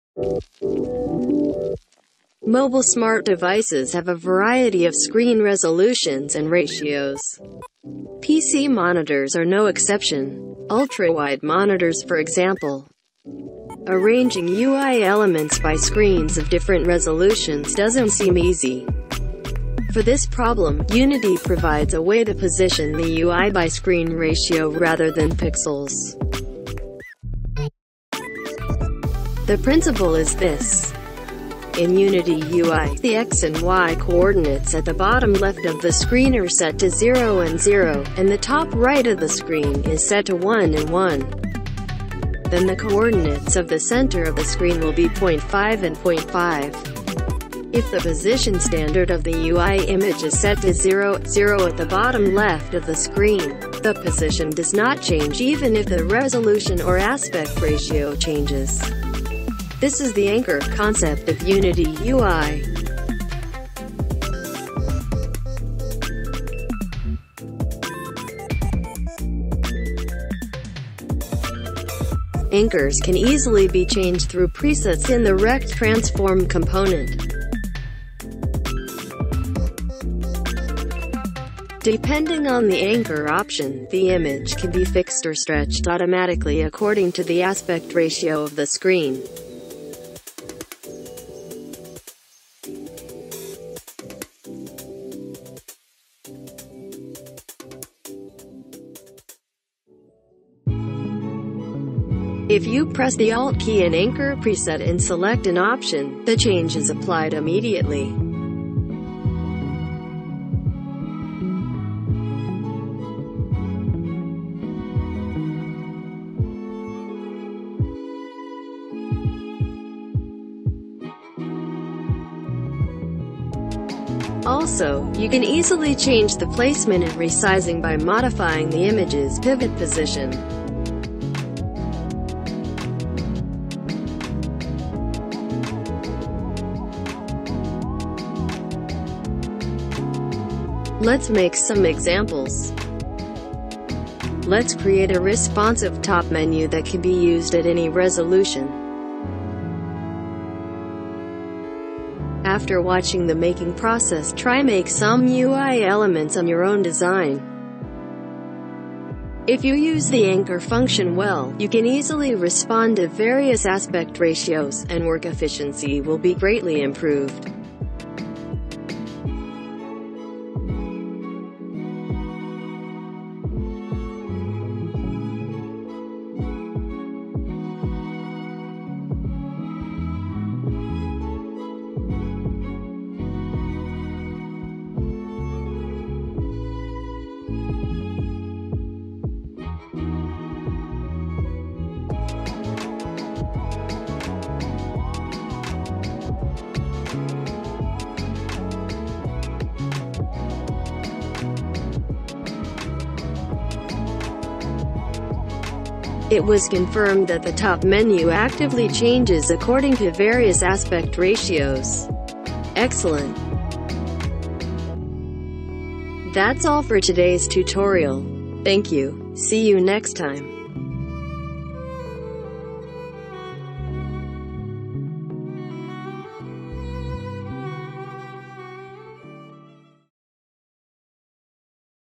Unity UI : How to set UI for various screen ratios
Hard to understand about Unity UI?
If you understand the anchor of Unity UI, you can quickly and easily work on various screen sizes.
I will solve your UI concerns with simple principles and short examples of Unity UI.